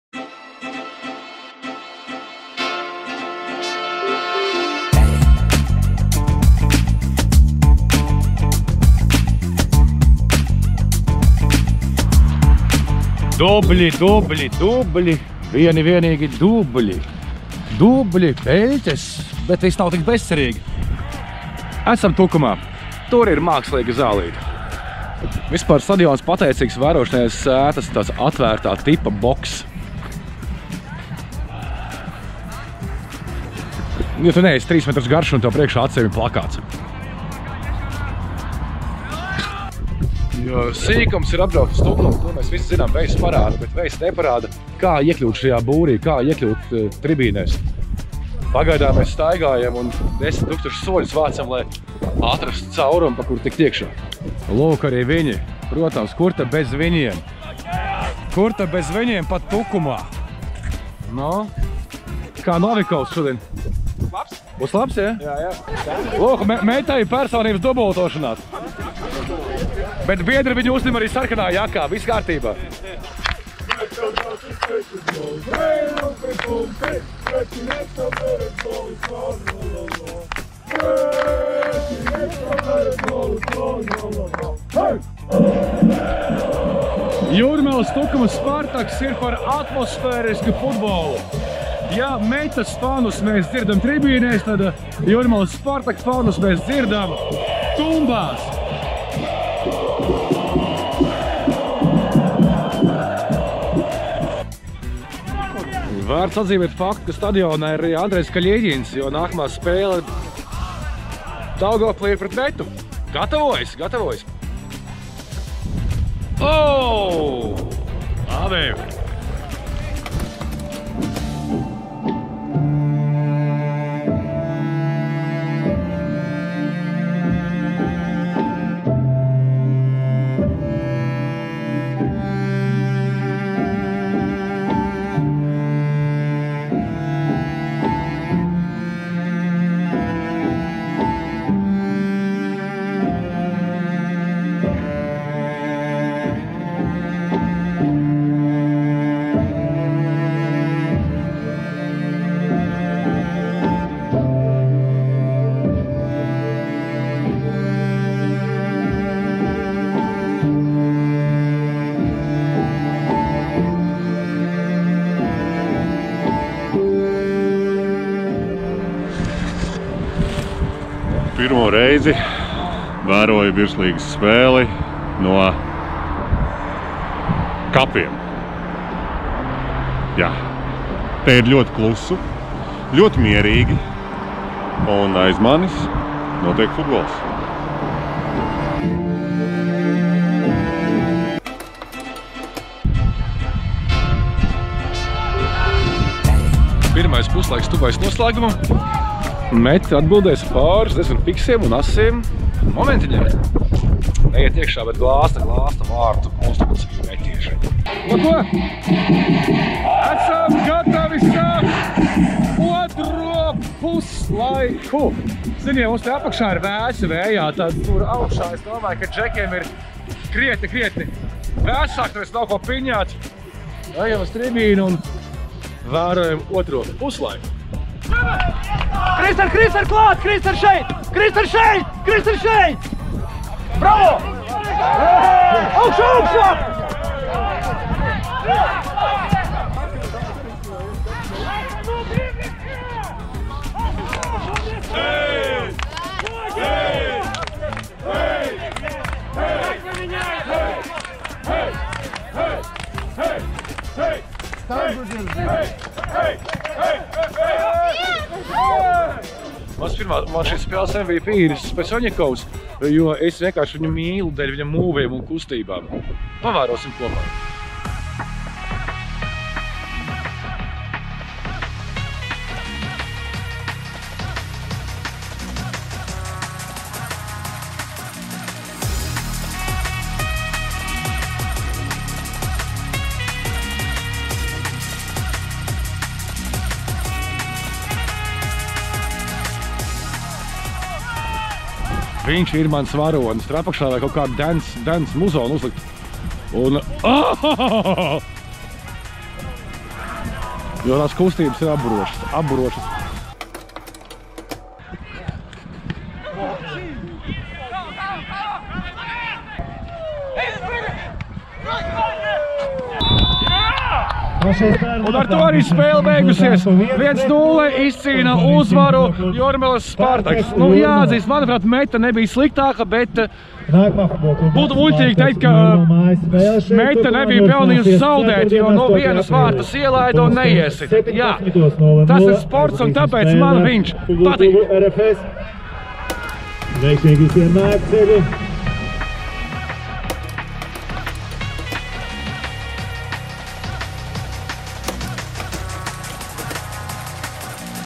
Futbols Tukumā
Mini reportāža no Virslīgas 2. kārtās spēles starp FK Spartaks Vs. FK Metta. Ielūkojāmies kā ir vērot spēli esot tribīnēs un aiz sētas.

Patika saturs un gribi vēl šādus līdzīgus video mūsu izpildījumā?